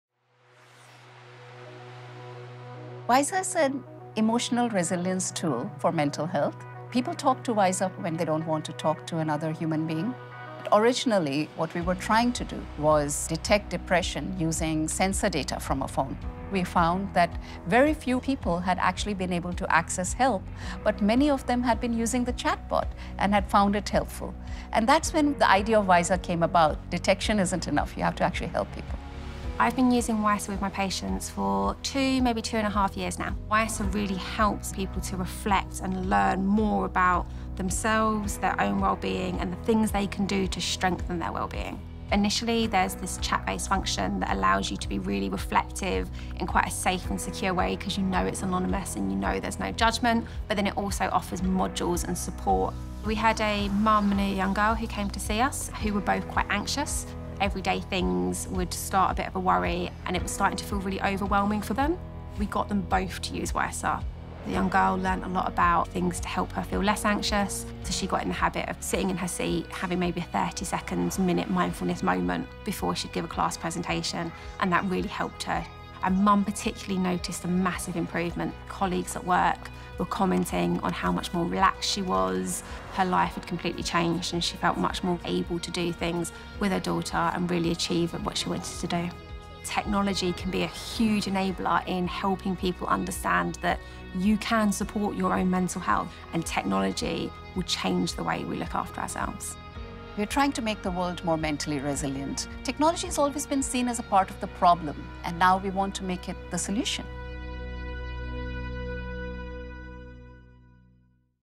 Building mental resilience through technology
Hear from Jo Aggarwal, co-founder and CEO of Wysa, and psychiatric nurse Emma Selby, on how technology can be used to improve mental health and build mental resilience.